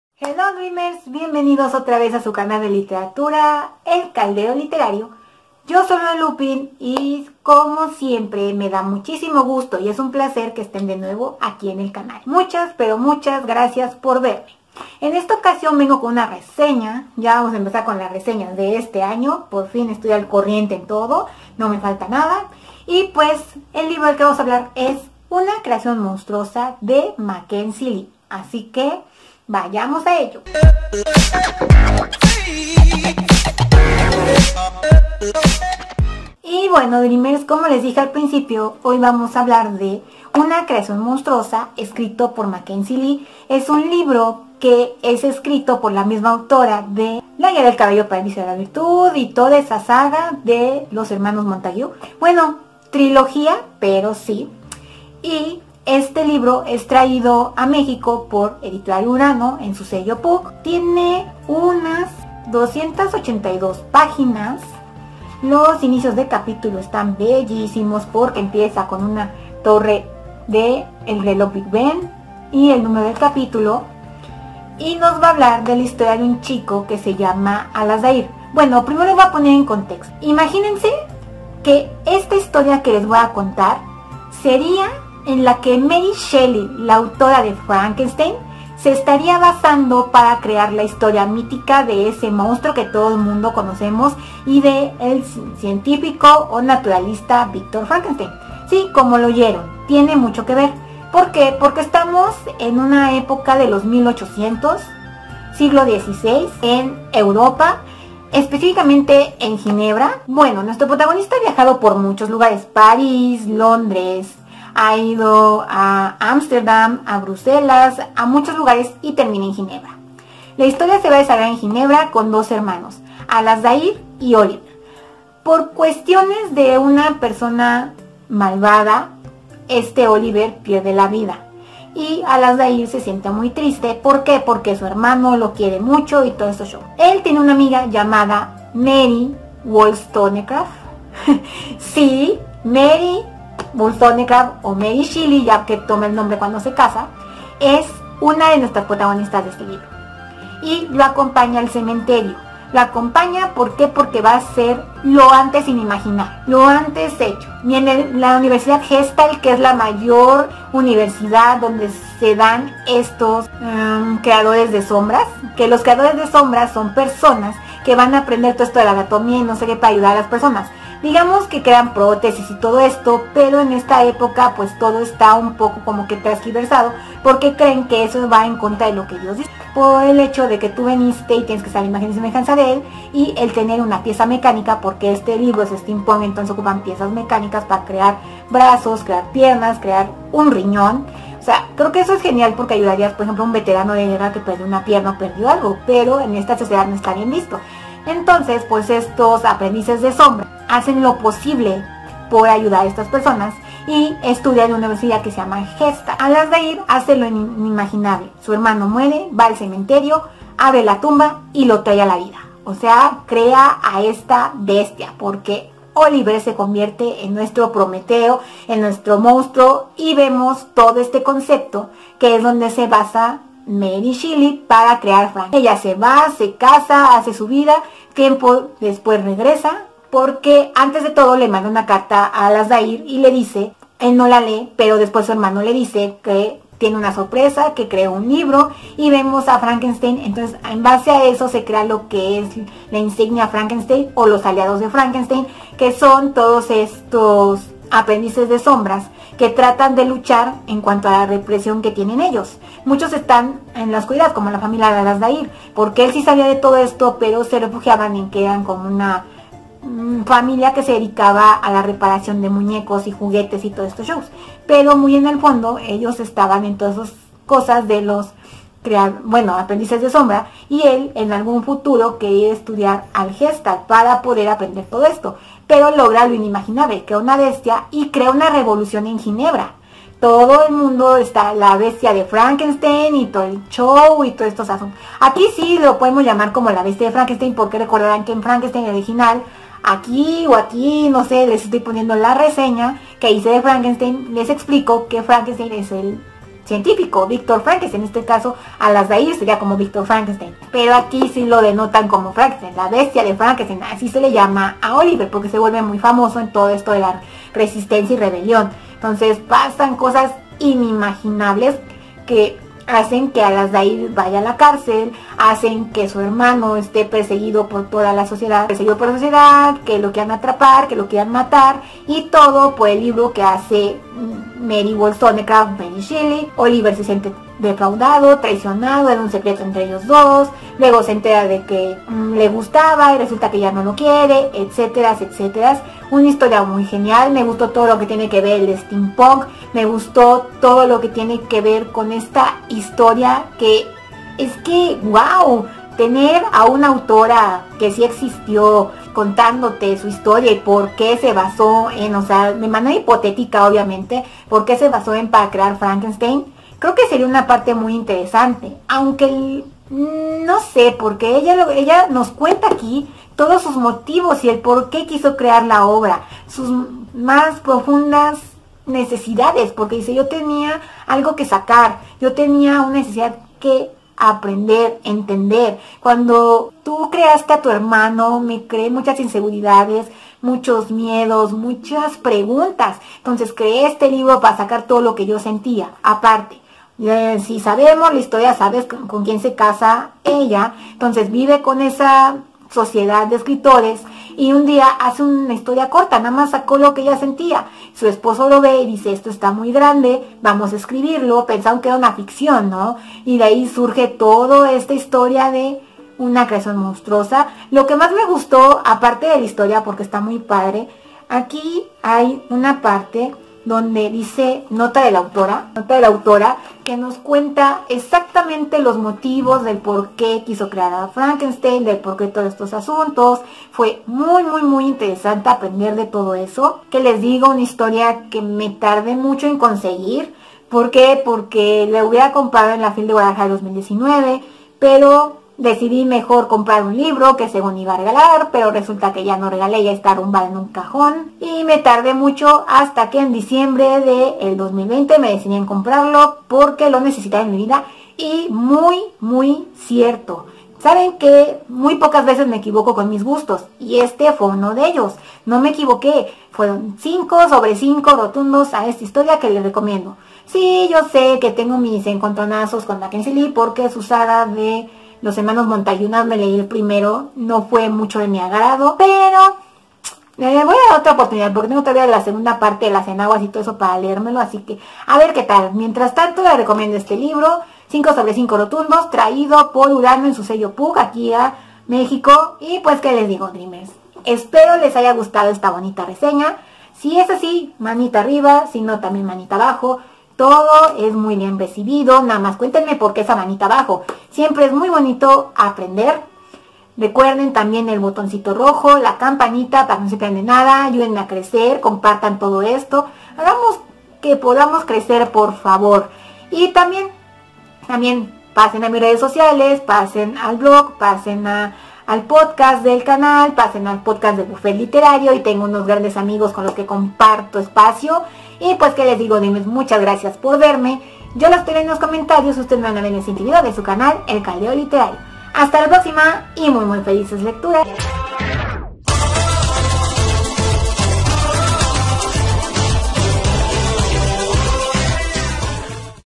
Hablemos De...Un libro que podría ser la historia verdadera de Frankenstein
No se olviden que todos los MIÉRCOLES Y SÁBADO hay nuevo vídeo.

Si, vamos a hablar de un libro, ya era justo y necesario, este libro es el que podría a ver sido en el que Marie Shelley se basara para crear Frankenstein su iconica obra, ¿Que pensarían de eso?

✭☆✧✭☆ PULSA EN MOSTRAR MÁS ✭☆✧✭☆

- FELICES LECTURAS -
- ¡ LA LECTURA ES VIDA!

Lorelupin =)